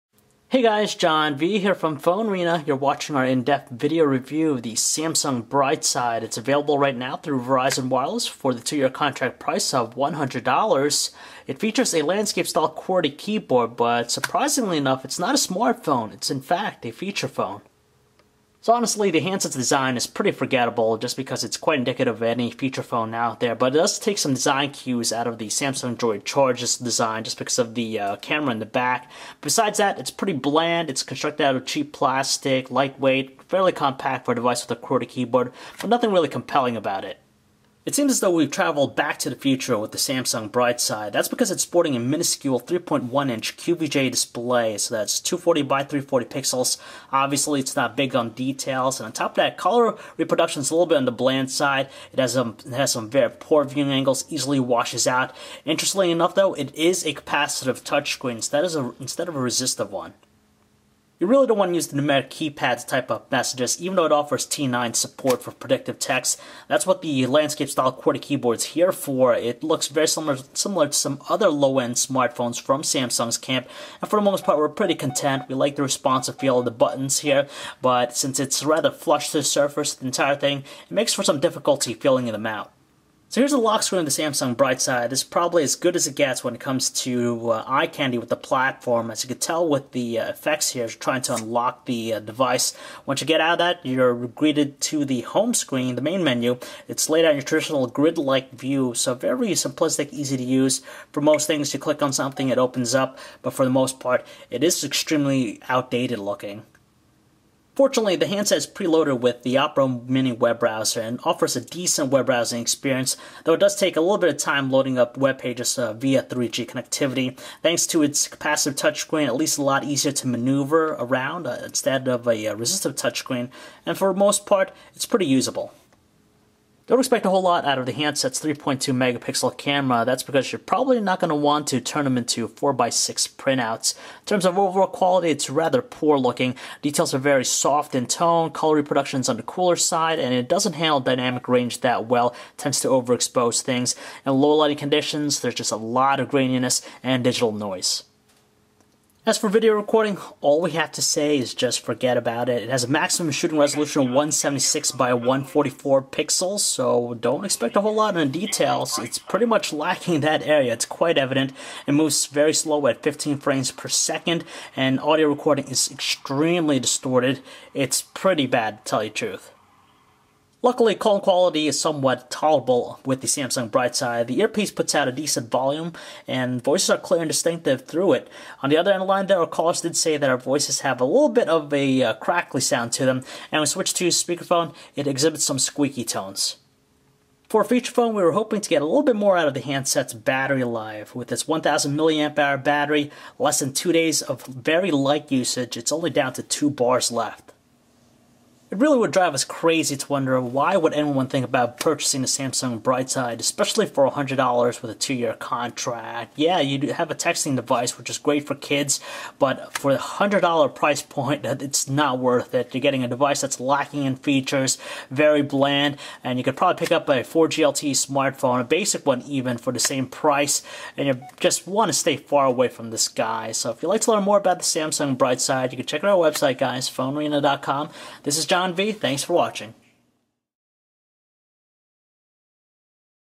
Samsung Brightside Review
PhoneArena reviews the Samsung Brightside. Oh smartphones, it's very easy for us to be jaded by them nowadays, especially when companies pump them out of the factories at a prolific rate. In hindsight, we realize why they're so prevalent in our society, but for those who still long for yesterday's feature phones, there's the Samsung Brightside for Verizon here to the rescue for those who primarily text...